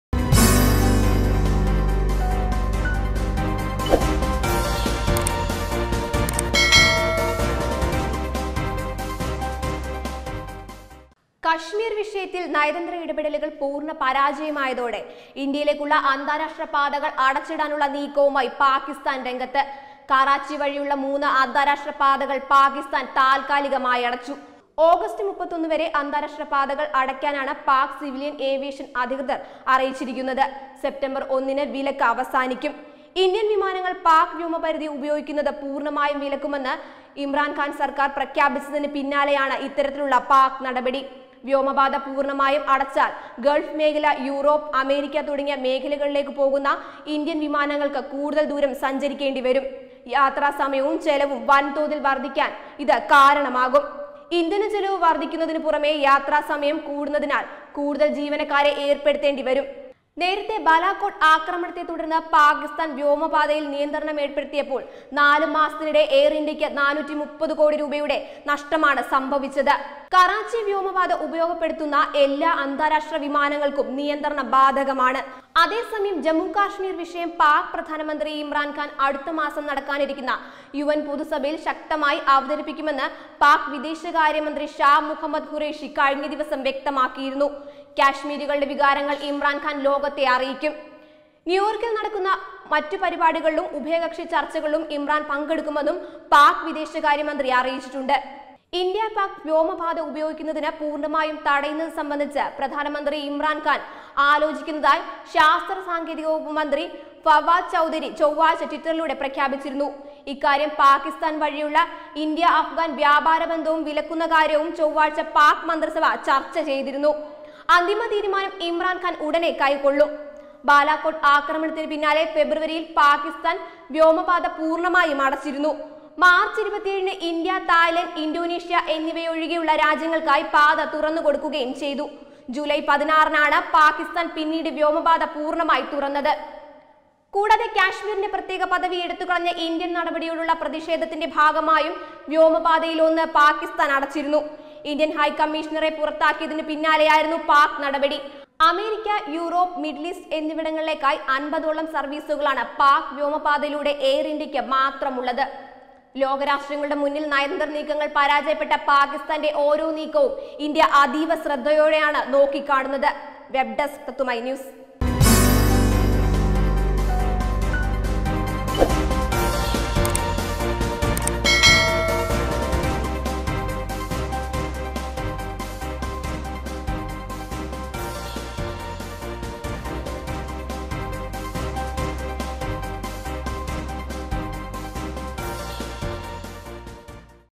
അരിശം തീരുന്നില്ല : വിവേകമല്ല ഇത് വിവരക്കേട്
കശ്മീര്‍ വിഷയത്തില്‍ നയതന്ത്ര ഇടപെടലുകള്‍ പൂര്‍ണ പരാജയമായതോടെ ഇന്ത്യയിലേക്കുള്ള അന്താരാഷ്ട്ര പാതകള്‍ അടച്ചിടാനുള്ളനീക്കവുമായി പാകിസ്താന്‍ രംഗത്ത്.കറാച്ചി വഴിയുള്ള മൂന്ന് അന്താരാഷ്ട്ര പാതകൾ പാക്കിസ്ഥാൻ താത്കാലികമായി അടച്ചു.  ആഗസ്റ്റ് 31 വരെ അന്താരാഷ്ട്ര പാതകൾ പാക് സിവില്‍ ഏവിയേഷന്‍ അധികൃതര്‍ അറിയിച്ചിട്ടുണ്ട്.സെപ്റ്റംബർ ഒന്നിന് വിലക്ക് അവസാനിക്കും.ഇന്ത്യൻ വിമാനങ്ങൾ പാക് വ്യോമപരിധി ഉപയോഗിക്കുന്നത് പൂർണമായും വിലക്കുമെന്ന് ഇംറാന്‍ സർക്കാർ പ്രഖ്യാപിച്ചതിന് പിന്നാലെയാണ് ഇത്തരത്തിലുള്ള പാക് നടപടി